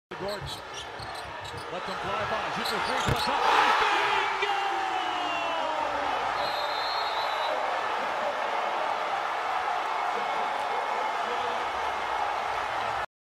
Eric Gordon forces overtime vs Wizards!
Eric Gordon Knocks Down This Triple To Send The Game To Overtime